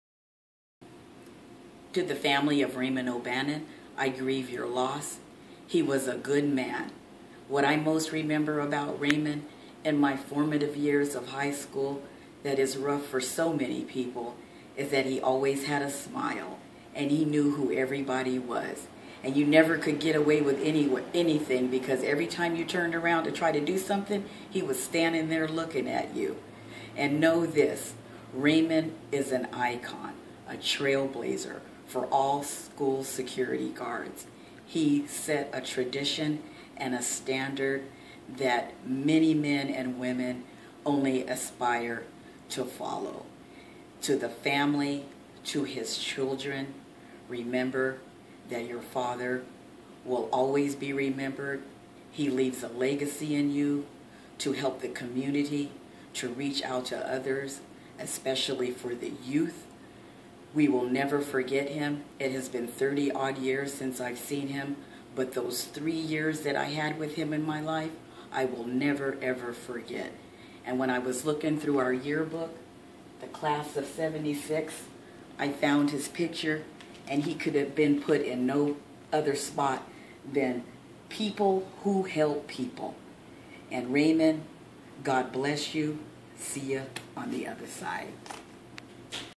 Raymond O'Bannon Message
A message to the family of O'BANNON, Sr., Raymond 'BIG RAY'
Raymond O'Bannon, a long-time employee of Grant Unified School District/Twin Rivers, passed away on July 20, 2013. He was my high school sec